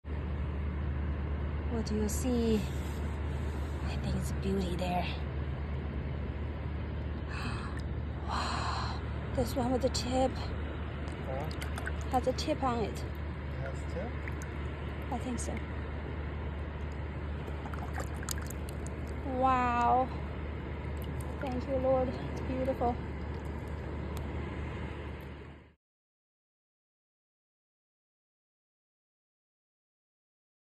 Finding shelling gold in the 10 thousand islands
Night shelling Low tide on keewadin Island